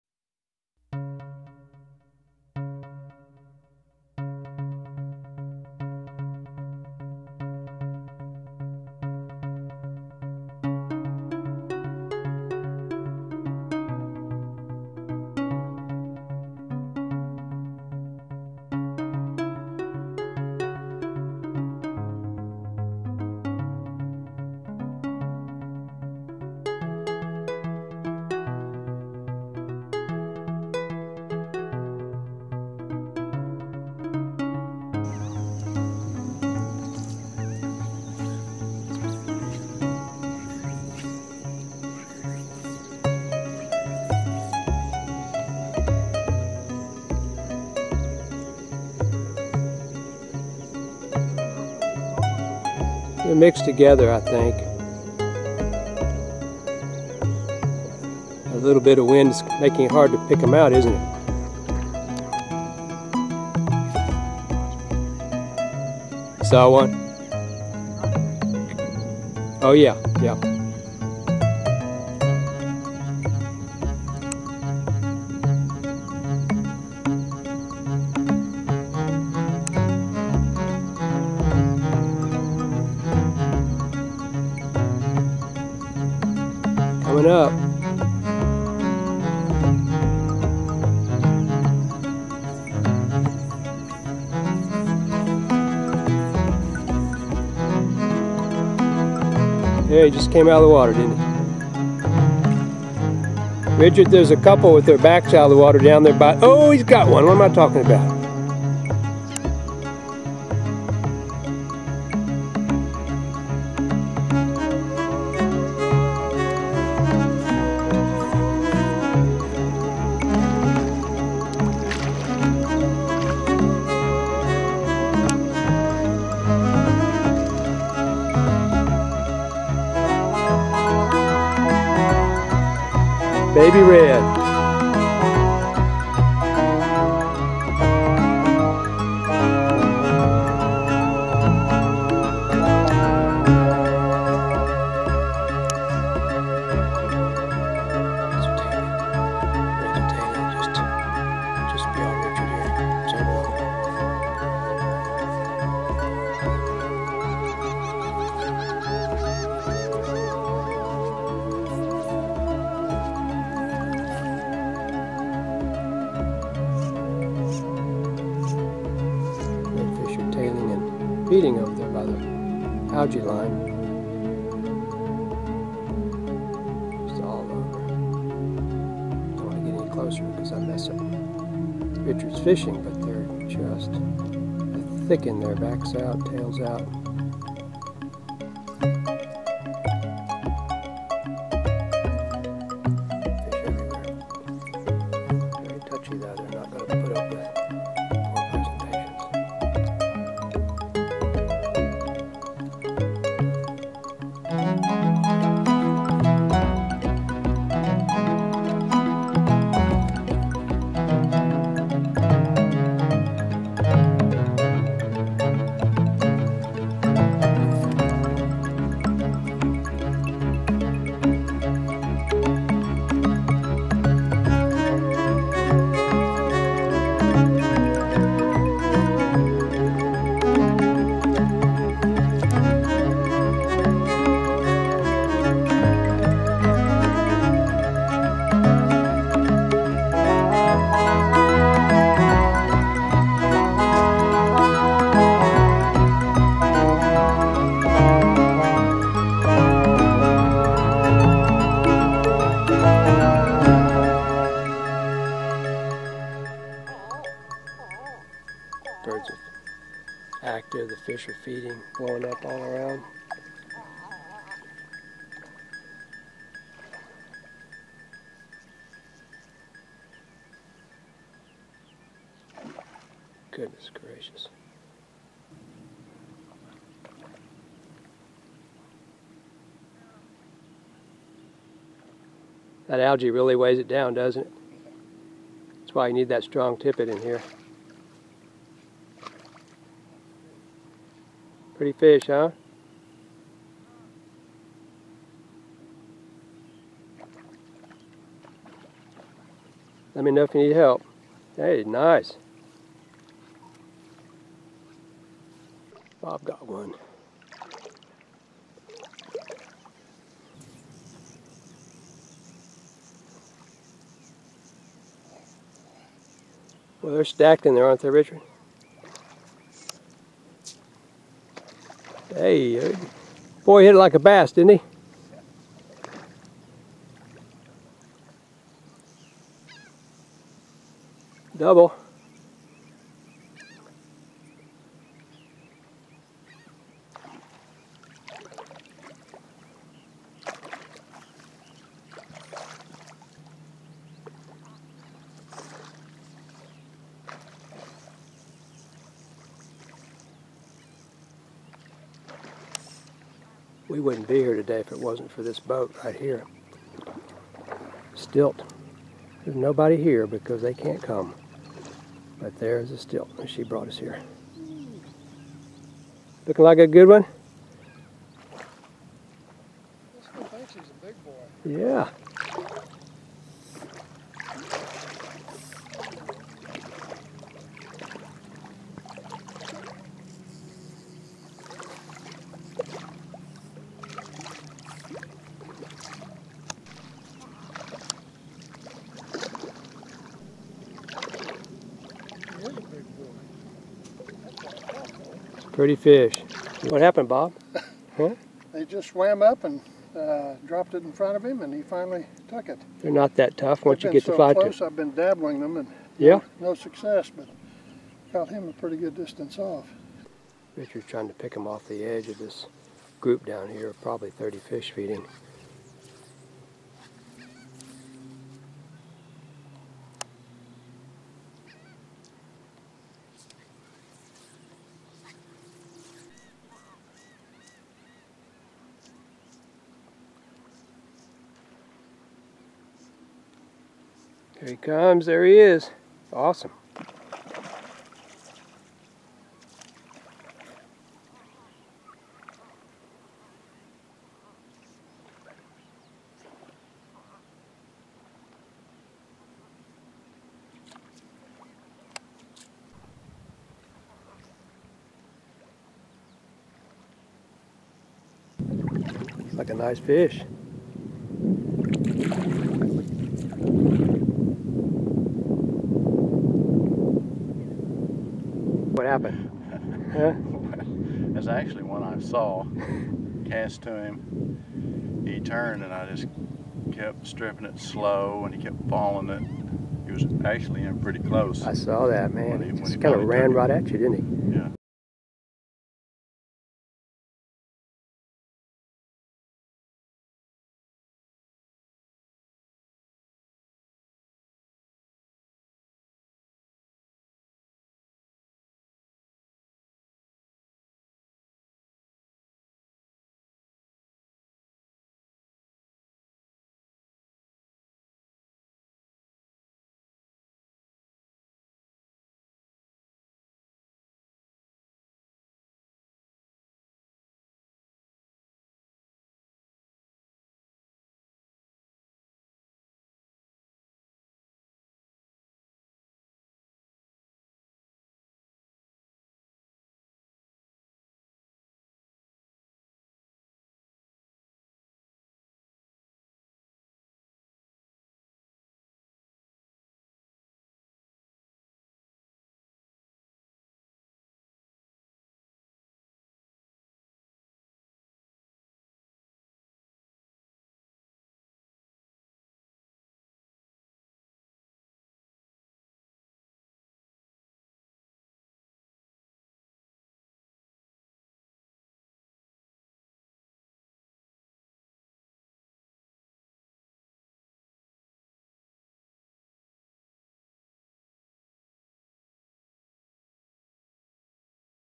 Fly Fishing for Tailing Redfish
I guided Richard Back of Arkansas with his buddy Bob in one of our finest early morning venues. The roseate spoonbills and a variety of egrets were feeding alongside as the guys casted to tailing and "backing" redfish.